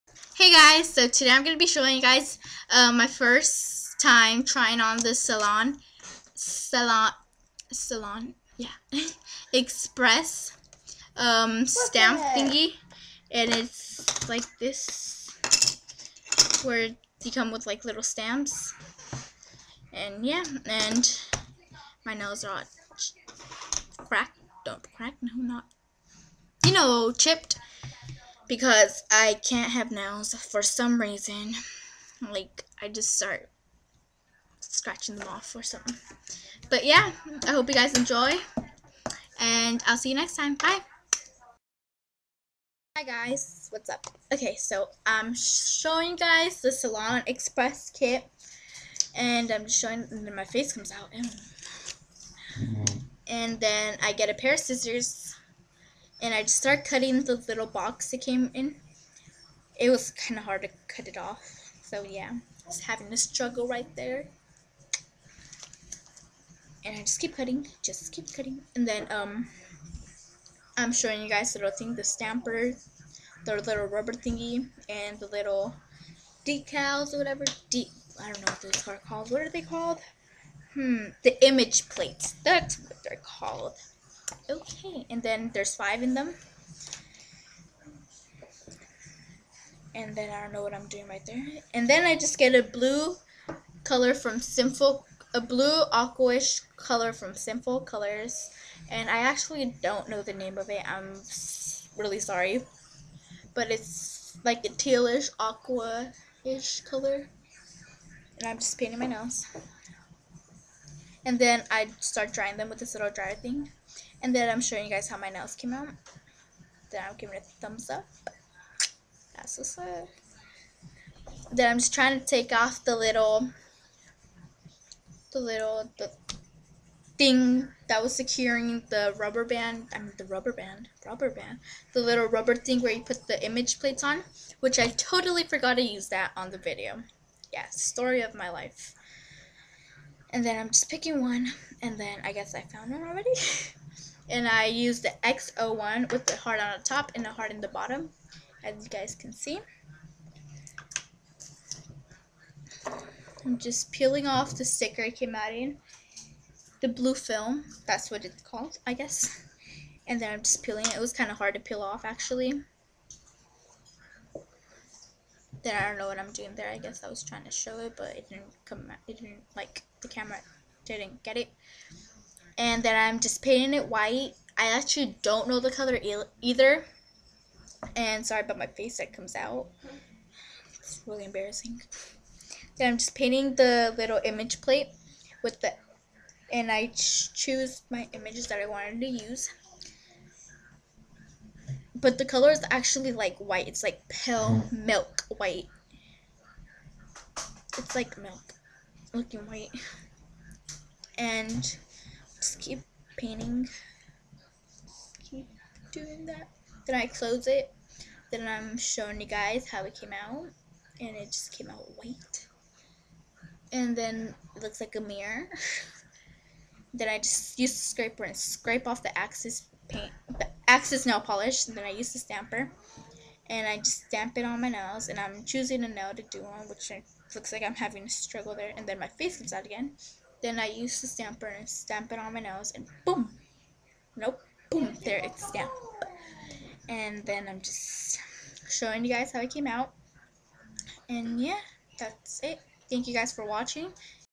trying the salon express nail stamps for the first time
whats up your  girl mayra here okay so today I'm showing you guys how the salon express nail came out on me hope you guys enjoy and I hope this helped you guys out if your trying to buy it love you guys.